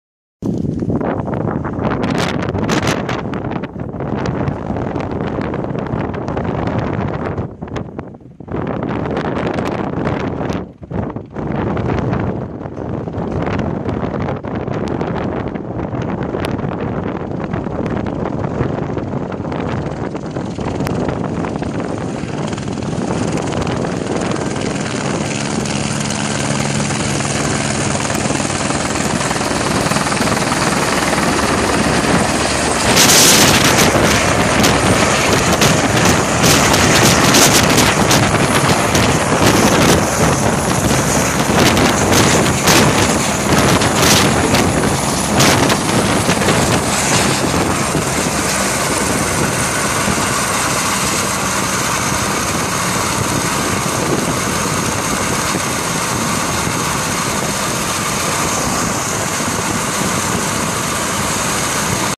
ZS-HDK AS350 B3 comes home
ZS-HDK AS350 B3 comes home for a little visit before moving off to its next job. It was short but so good to see her come and go.
Sorry for the shaking, i didnt realize the down wash was going to hit me that hard